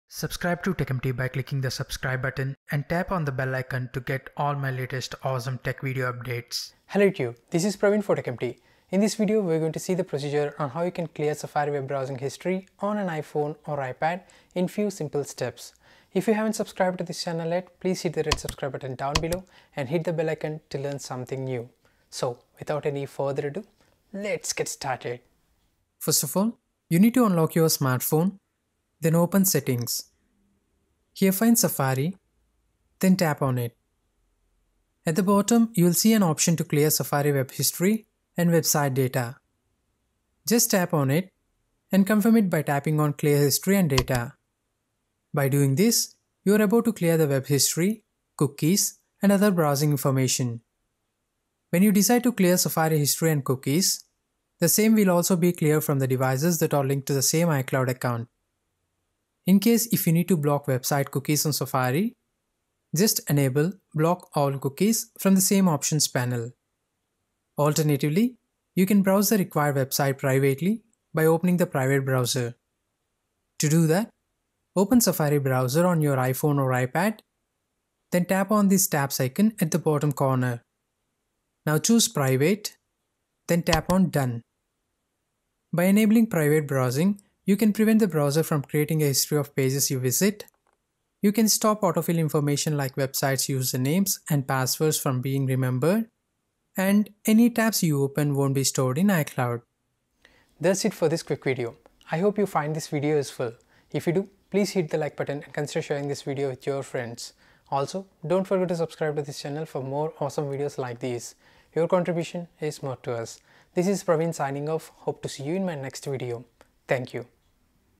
How To Clear Safari History & Cookies on an iPhone/iPad?
Learn the way to clear Safari Web History, Cookies and Cache on an iPhone or iPad. If you are using any version of iOS device, you can follow these steps to delete the web history of Safari browser.

The whole process takes just 30 seconds. If you don't know about the process of clearing website history, cookies and cache of Safari browser on iPhone then this is the time to know about it. Enhance your privacy and stay safe with the intruders.

First of all, you need to unlock your Smartphone then open settings.

Here find Safari then tap on it.

At the bottom you will see an option to clear Safari Web History and Website Data.

Just Tap on it and confirm it by taping on Clear History and Data. By doing this, you are about to clear the web history, cookies and other browsing information.

When you decide to clear Safari history and cookies, the same will also be cleared from the devices that are linked to the same icloud account.

In case if you need to block website cookies on Safari, just enable ‘Block All Cookies’ from the same options panel.

Alternatively, you can browse the required websites privately by opening the private browser.

To do that, open Safari browser on your iPhone or iPad, then tap on this tabs icon at the bottom corner.

Now choose ‘Private’ then tap on ‘Done’.